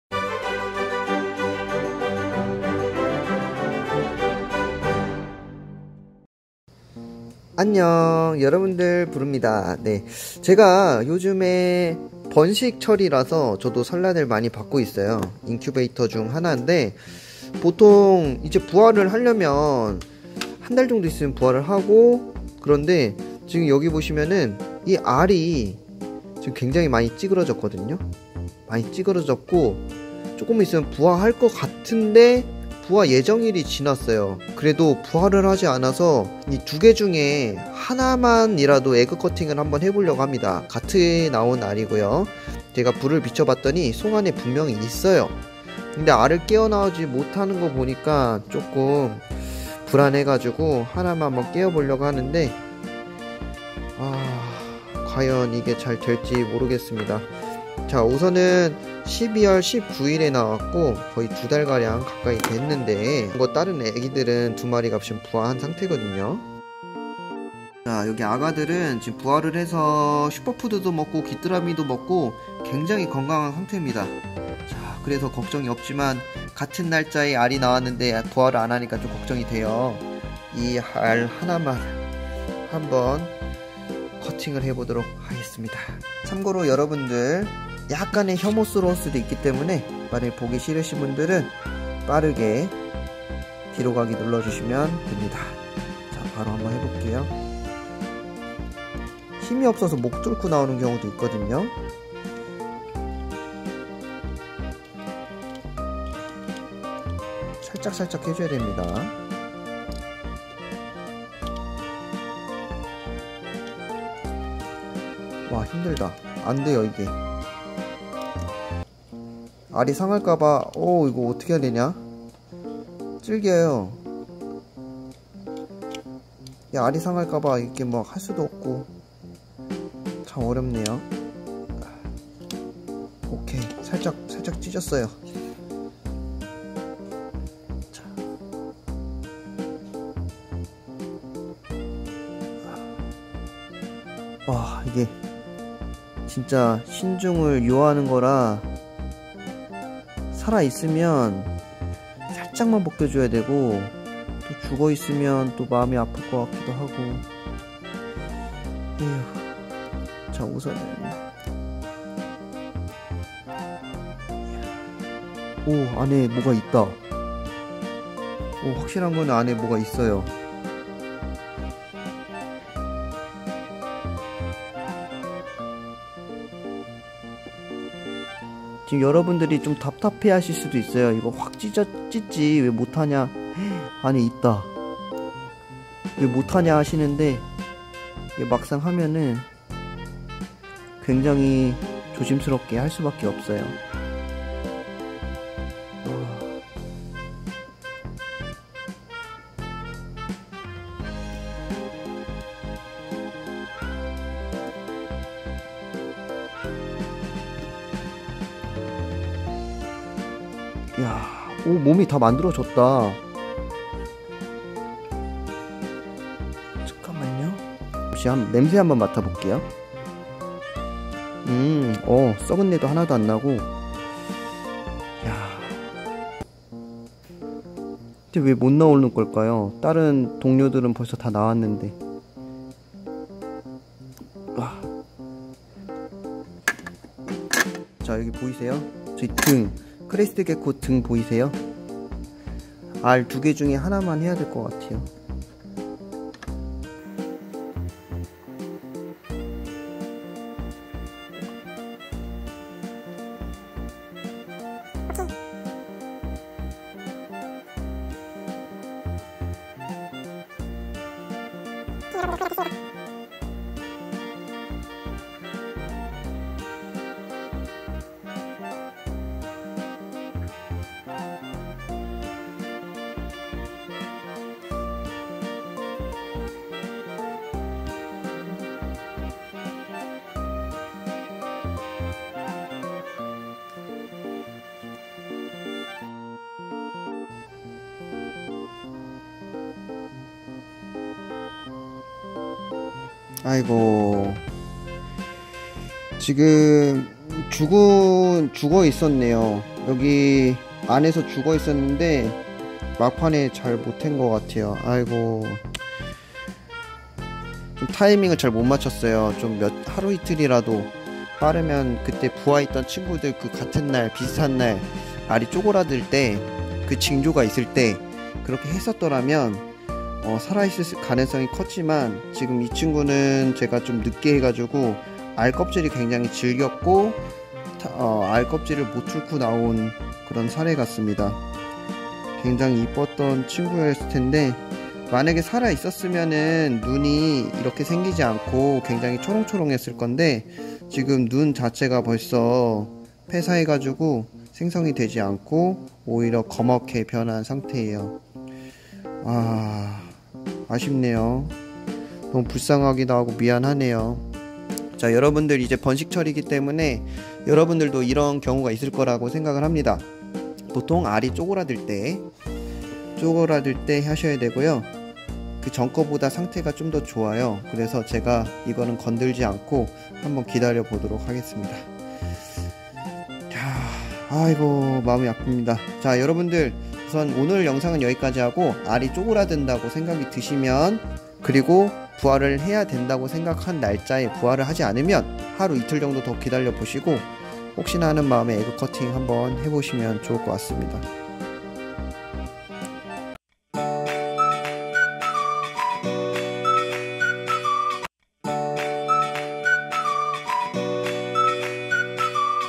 긴급) 알에서 못 태어나는 도마뱀...살릴수 있을까...? [정브르]
설명을 꼼꼼하게 읽어보시고, 괜히 건강한 컷팅하지 마시고, 저와같이 동배개체들은 다 깨어나고 잘 태어나고 , 알이 태어날 징조, 날짜가 보이는데 안 태어났을때 타이밍 잘맞춰서 하는것입니다.!

이번에는 안 좋게 되었지만 , 여러분들도 이 영상을 보시고 좋은 결과 얻었으면 좋겠습니다!